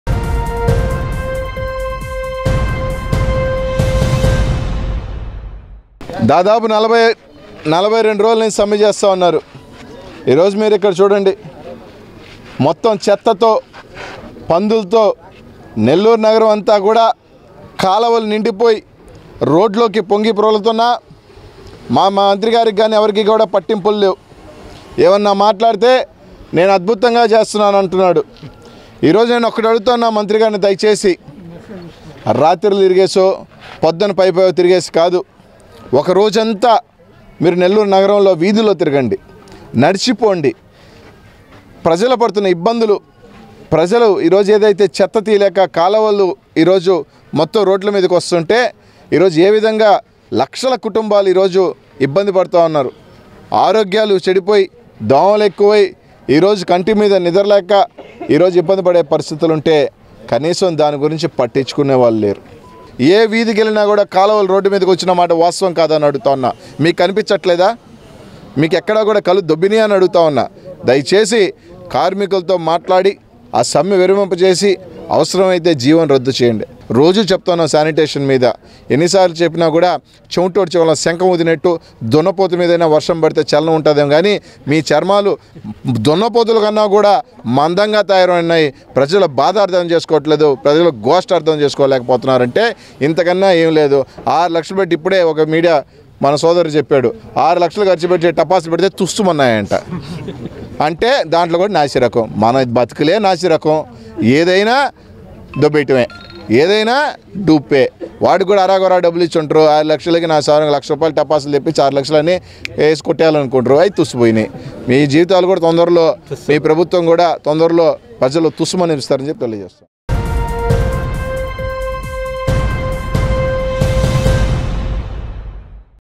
తొందర్లోనే తుస్.. - NDN News
తొందర్లోనే తుస్..
దర్గాలో టపాసులు తుస్ అన్నట్టుగానే తొందర్లోనే టీడీపీ కూడా ఎన్నికల్లో తుస్ అంటుందని జోస్యం చెప్పారు సిటీ ఎమ్మెల్యే అనిల్ కుమార్ యాదవ్. నెల్లూరు కార్పొరేషన్ కార్మికులు చేస్తున్న సమ్మెను మంత్రి నారాయణ, అధికార పక్ష నేతలు పట్టించుకోలేదని ధ్వజమెత్తారు. కమీషన్ల కోసమే మంత్రి నారాయణ కార్మికుల సమస్యను పరిష్కరించేందుకు చొరవ చూపడం లేదని విమర్శించారు.